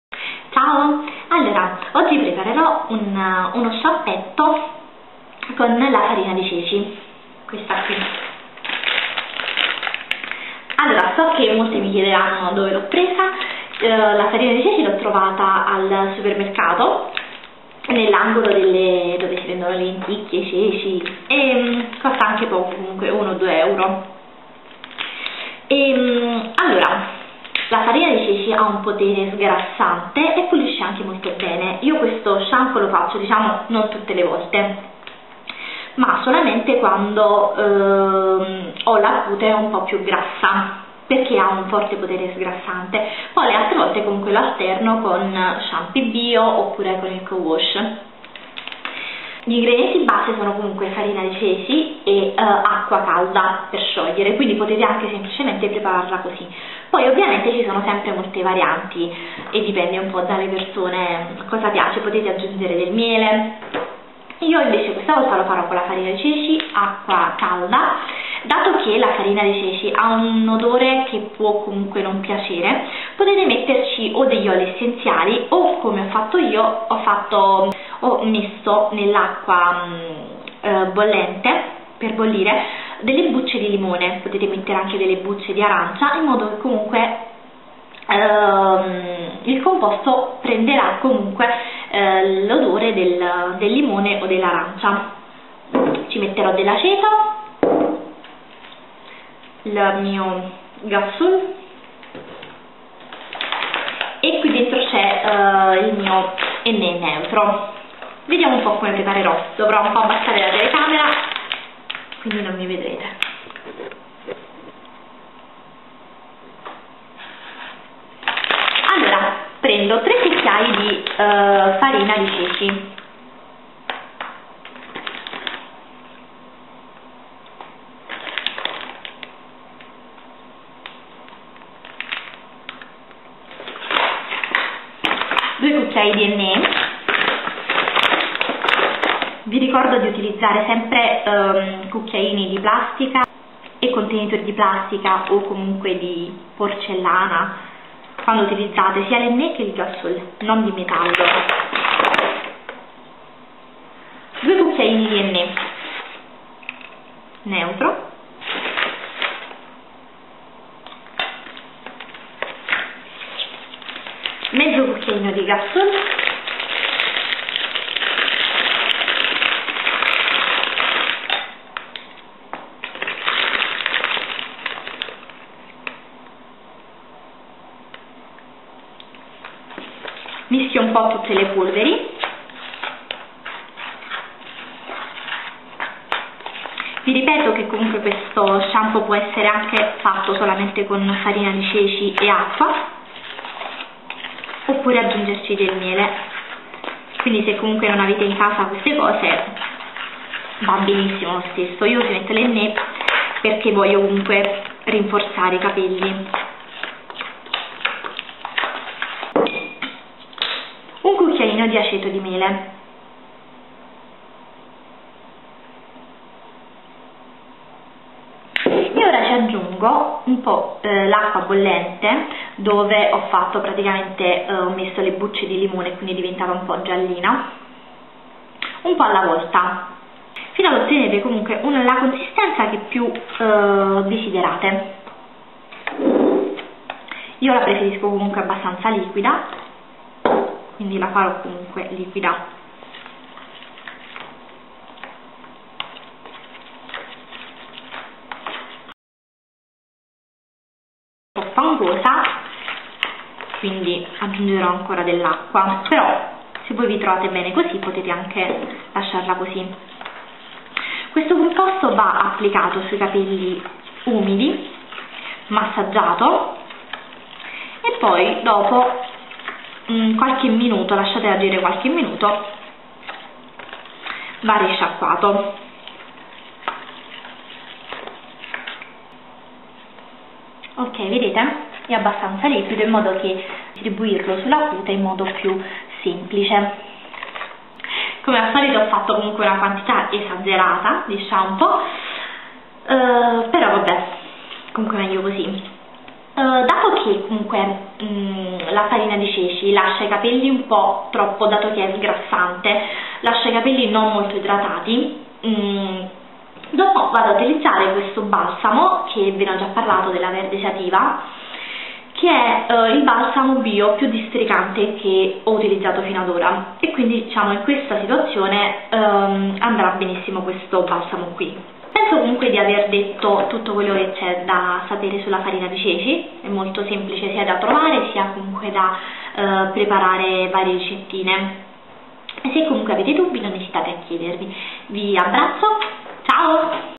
shampoo con farina di ceci...
gli ingredienti che ho utilizzato sono Farina di ceci, acqua bollente con all'interno bucce di limone, hennè neutro, ghassoul ed aceto di mele, ma se preferite potete anche farlo semplice o con del miele.
vi abbraccio.